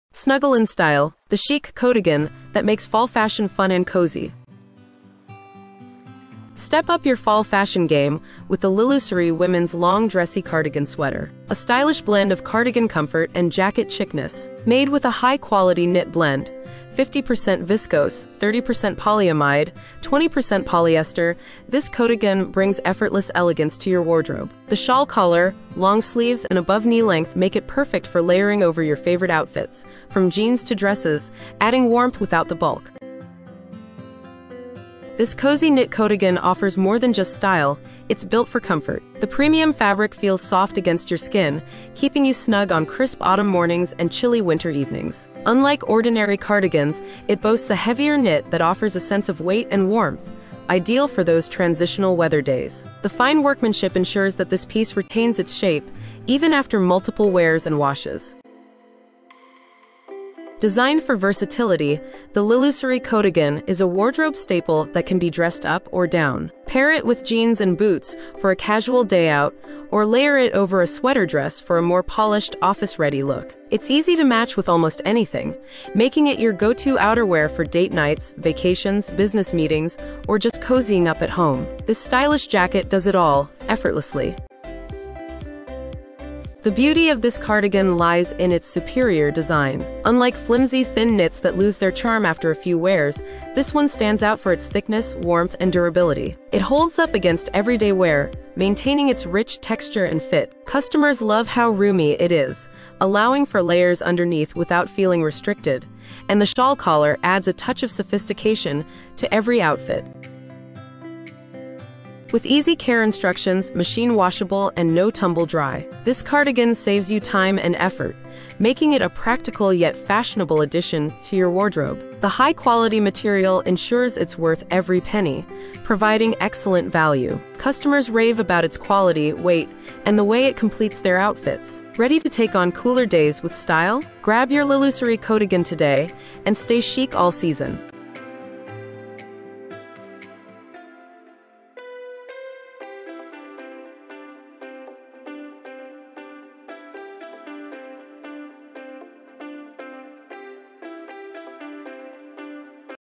Snuggle in Style: The Chic Coatigan That Makes Fall Fashion Fun & Cozy!
Step up your fall fashion game with the LILLUSORY Women's Long Dressy Cardigan Sweater – a stylish blend of cardigan comfort and jacket chicness. Made with a high-quality knit blend (50% viscose, 30% polyamide, 20% polyester), this coatigan brings effortless elegance to your wardrobe. The shawl collar, long sleeves, and above-knee length make it perfect for layering over your favorite outfits, from jeans to dresses, adding warmth without the bulk.

This cozy knit coatigan offers more than just style; it's built for comfort. The premium fabric feels soft against your skin, keeping you snug on crisp autumn mornings and chilly winter evenings. Unlike ordinary cardigans, it boasts a heavier knit that offers a sense of weight and warmth, ideal for those transitional weather days. The fine workmanship ensures that this piece retains its shape, even after multiple wears and washes.

Designed for versatility, the LILLUSORY coatigan is a wardrobe staple that can be dressed up or down. Pair it with jeans and boots for a casual day out, or layer it over a sweater dress for a more polished, office-ready look. It’s easy to match with almost anything, making it your go-to outerwear for date nights, vacations, business meetings, or just cozying up at home. This stylish jacket does it all, effortlessly.

The beauty of this cardigan lies in its superior design. Unlike flimsy, thin knits that lose their charm after a few wears, this one stands out for its thickness, warmth, and durability. It holds up against everyday wear, maintaining its rich texture and fit. Customers love how roomy it is, allowing for layers underneath without feeling restricted, and the shawl collar adds a touch of sophistication to every outfit.

With easy care instructions – machine washable and no tumble dry – this cardigan saves you time and effort, making it a practical yet fashionable addition to your wardrobe. The high-quality material ensures it’s worth every penny, providing excellent value. Customers rave about its quality, weight, and the way it completes their outfits. Ready to take on cooler days with style? Grab your LILLUSORY coatigan today and stay chic all season!